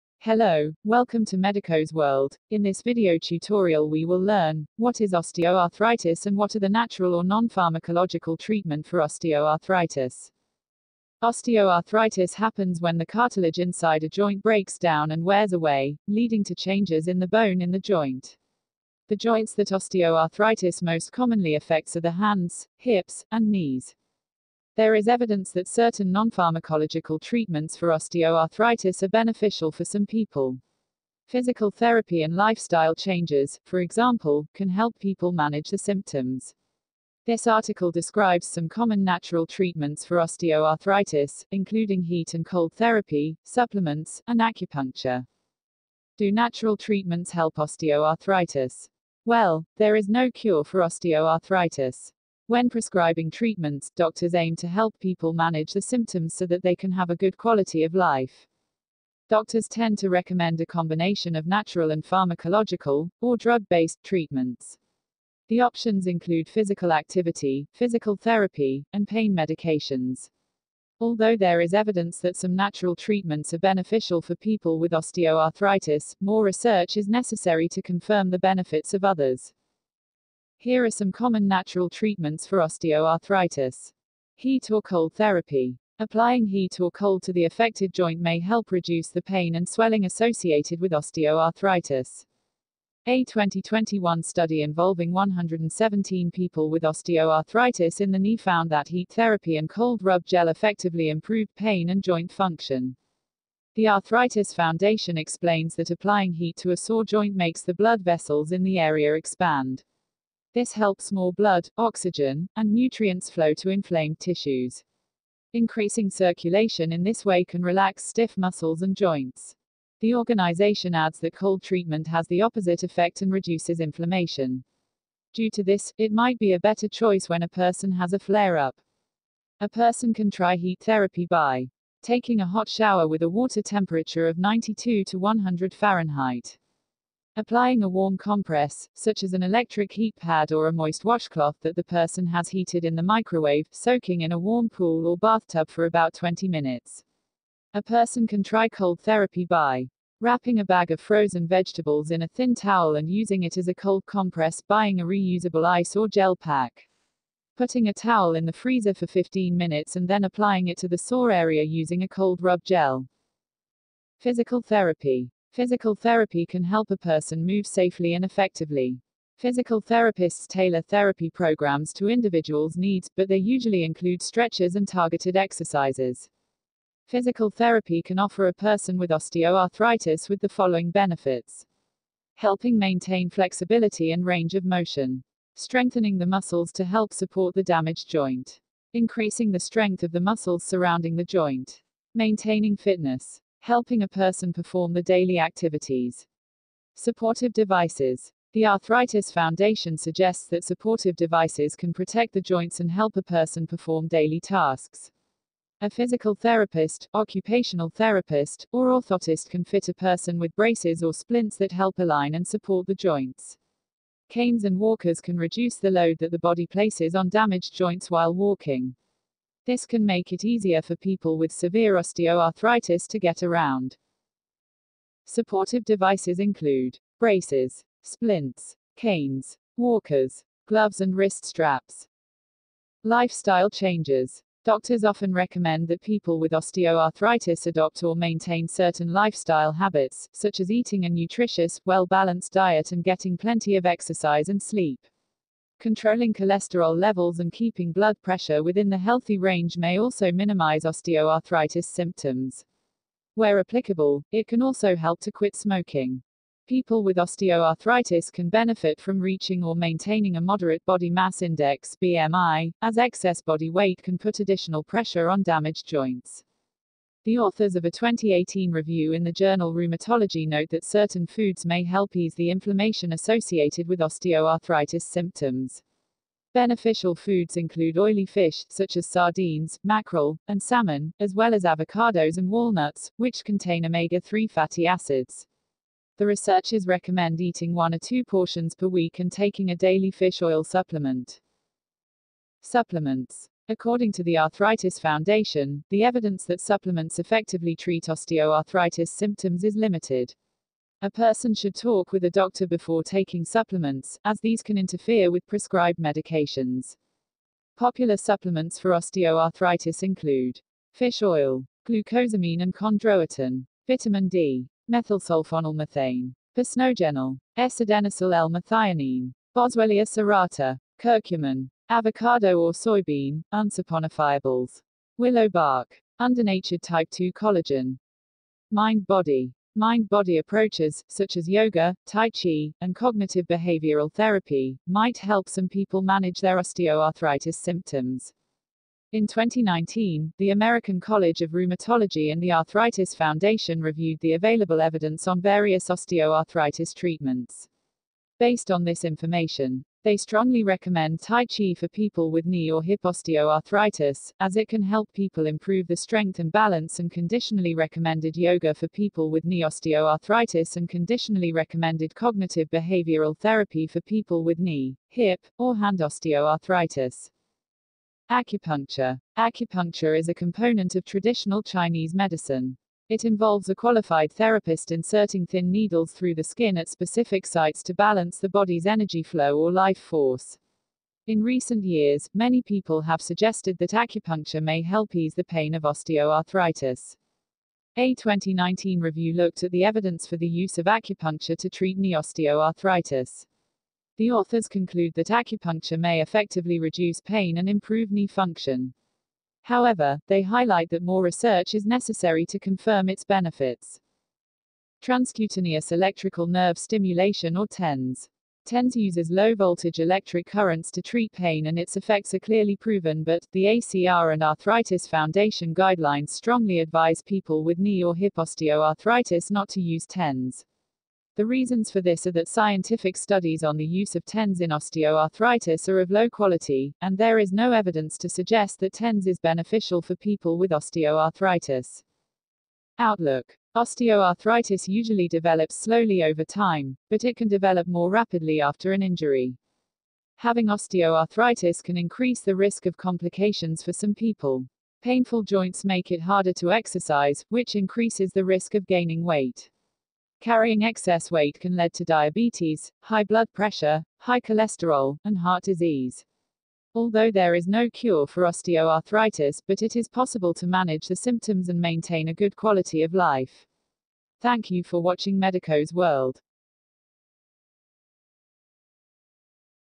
6 Natural Treatments - For Osteoarthritis
Osteoarthritis happens when the cartilage inside a joint breaks down and wears away, leading to changes in the bone in the joint. The joints that Osteoarthritis most commonly affects are the hands, hips, and knees.

There is evidence that certain nonpharmacological treatments for OA are beneficial for some people. Physical therapy and lifestyle changes can help people manage their symptoms.

This article describes some common natural treatments for OA, including heat and cold therapy, supplements, and acupuncture.